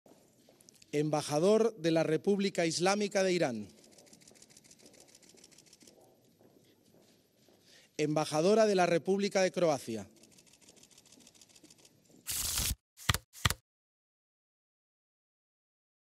El 'desaire' del embajador iraní a la reina Letizia al no poder darle la mano
El embajador de la República Islámica de Irán en España, Hassan Ghashghavi, ha protagonizado uno de los momentos más incómodos de la jornada al evitar el saludo a la reina Letizia en el Palacio Real, donde los monarcas han recibido al Cuerpo Diplomático en una de sus citas tradicionales al inicio del año.

La situación se ha producido cuando ha llegado el turno del diplomático para saludar a sus majestades. Ghashghavi ha estrechado la mano de Felipe VI, pero al llegar el turno de la monarca se ha llevado la mano al pecho y ha pasado de largo. Un gesto que ha inundado de comentarios las redes en lo que, para muchos, se ha convertido en un "desplante" por parte del diplomático iraní.